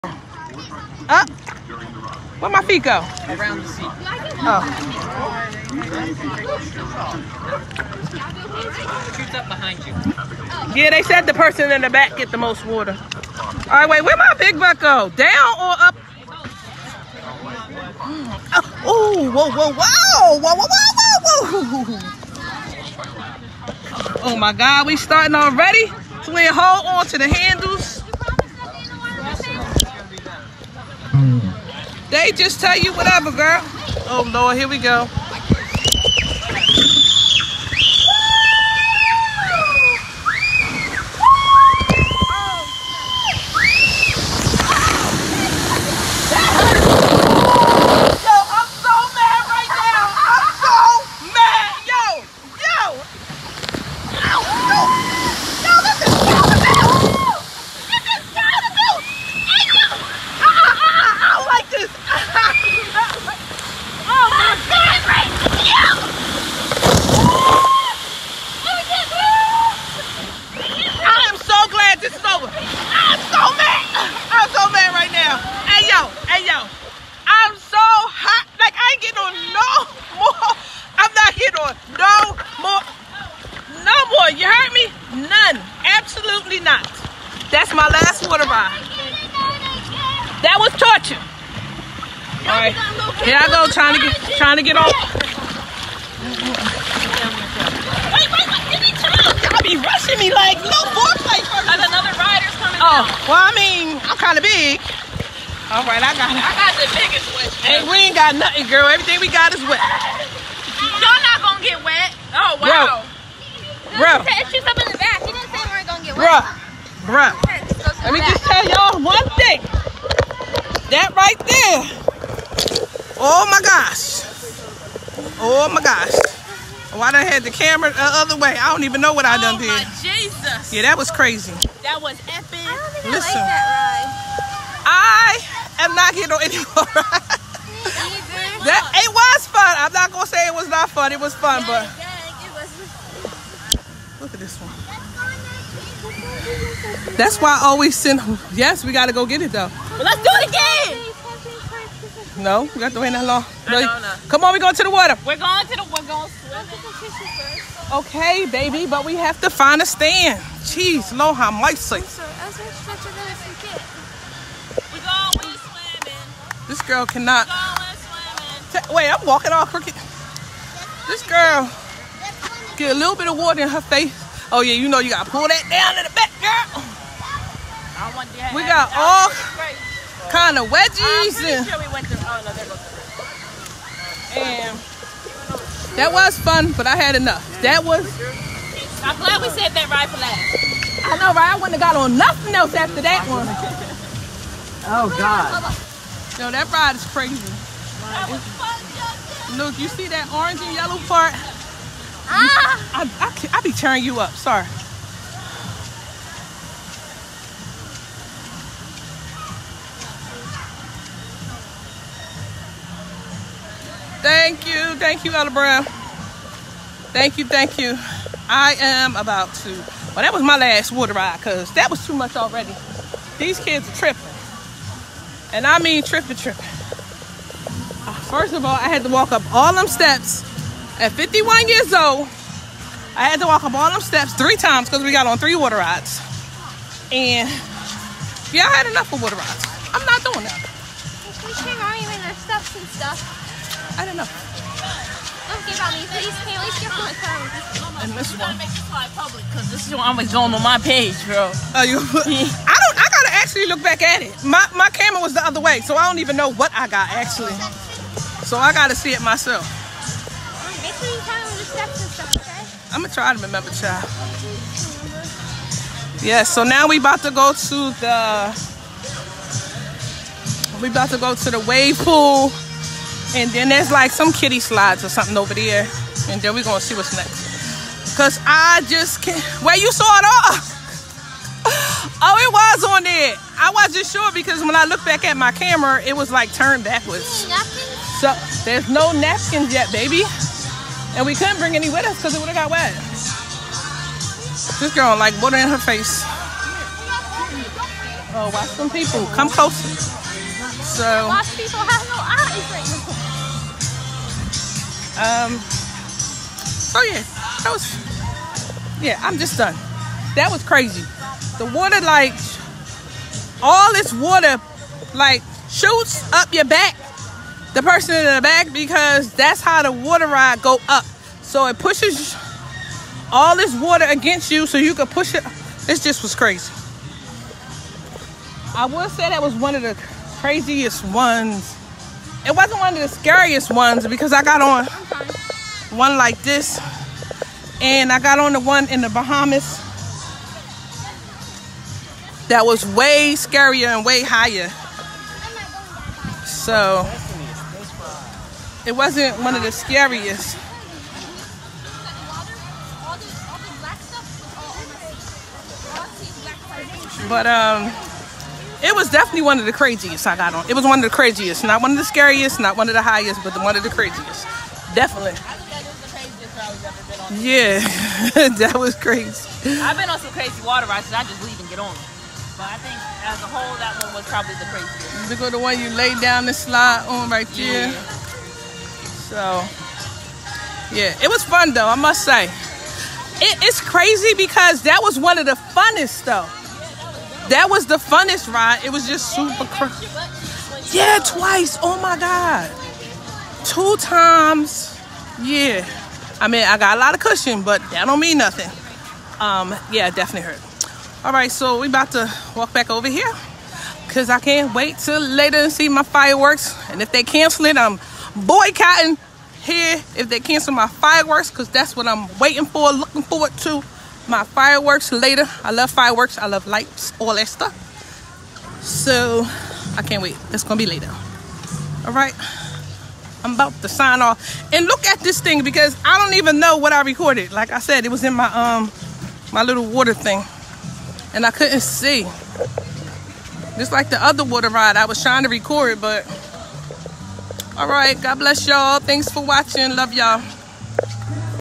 Me & my kids on a water ride in Orlando.
Crazy water ride with my children. And well I’m always loud.🤦🏽‍♀️😅